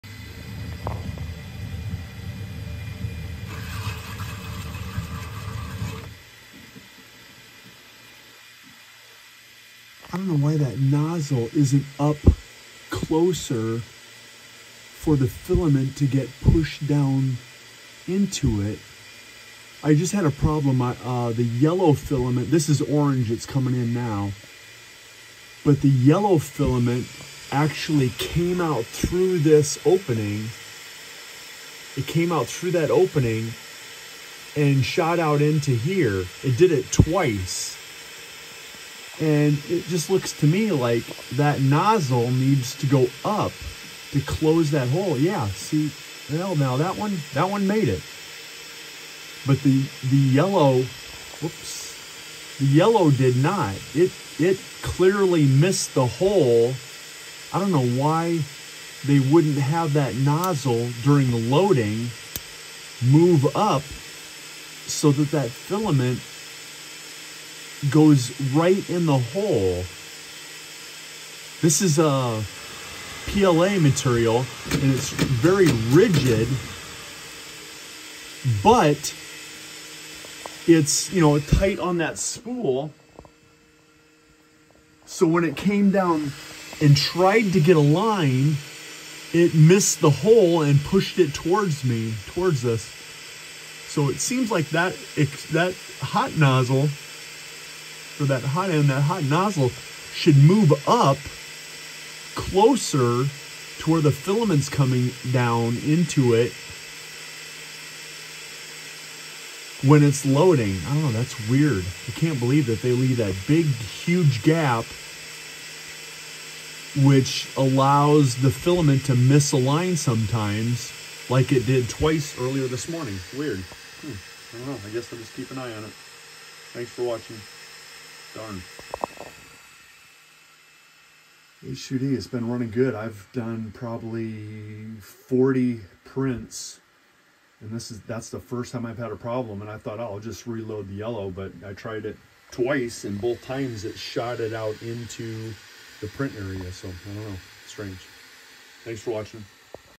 H2D Left nozzle Mis-feed - nozzle is in the lower position!?
4 out about 20 times - my H2D has Mis-fed PLA in the Left HotEnd/Nozzle extruder!?

During the initial’ 15-20 sec when the filament is just coming down into the nozzle - that head should be in the upper position to close that gap down - so the alignment of the filament is easier to auto-install….  then it would drop down and remove the tip cover - and continue it’s routine.

20% failure rate for AMS feed is not very good - especially for PLA.

They say to not run engineered material in the AMS… really!?…. I was planning to use about 60-70% engineered filament - in my two AMS units - UGH’…. Although I have had good so far - for sure in the Right Nozzle.